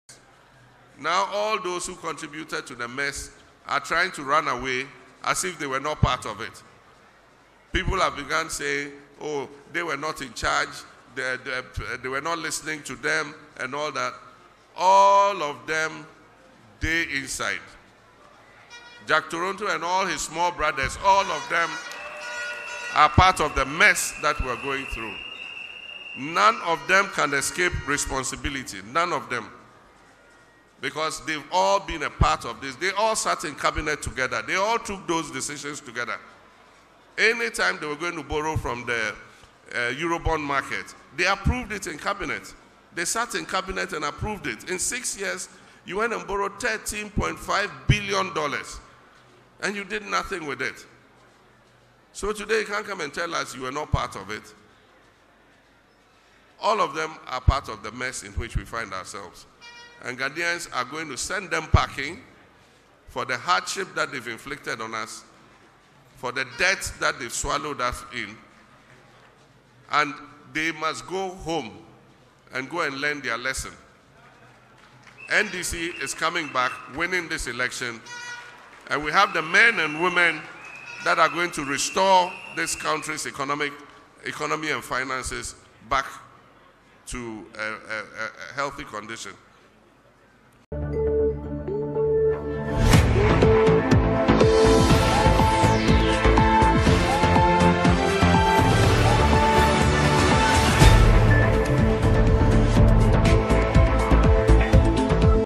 Ghana's economic challenges: No NPP member can be exonerated from current economic mess - Mahama
Ghana's economic challenges: No NPP member can be exonerated from current economic mess. - John Mahama